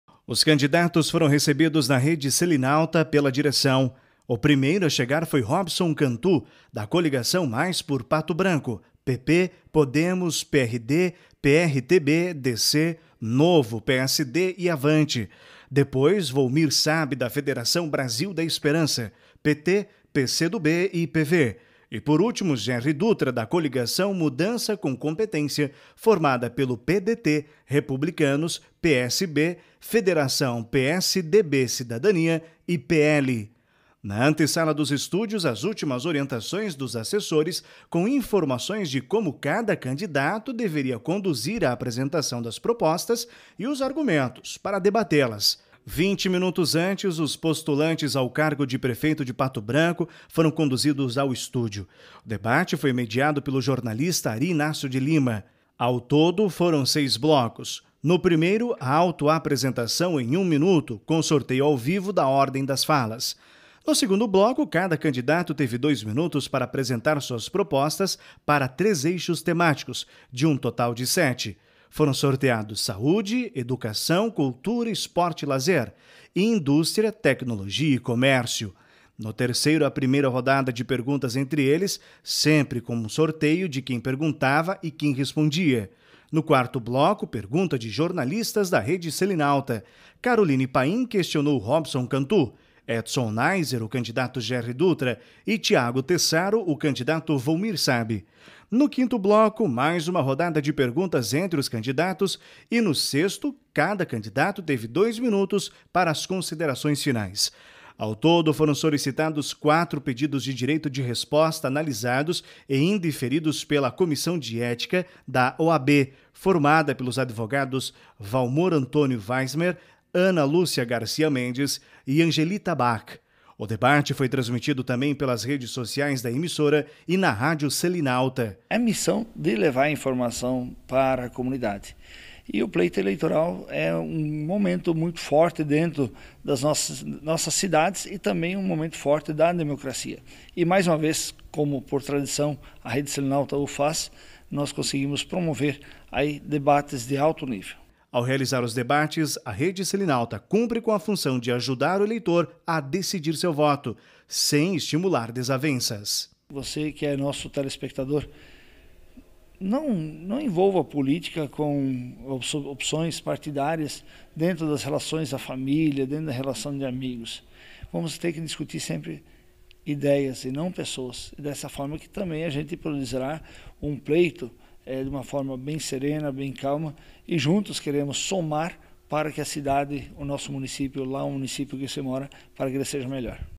TV Sudoeste e Rádio Celinauta realizam debate com os candidatos a prefeito de Pato Branco
Política: Os três candidatos a prefeito de Pato Branco participaram do debate realizado pela Rede Celinauta de Comunicação.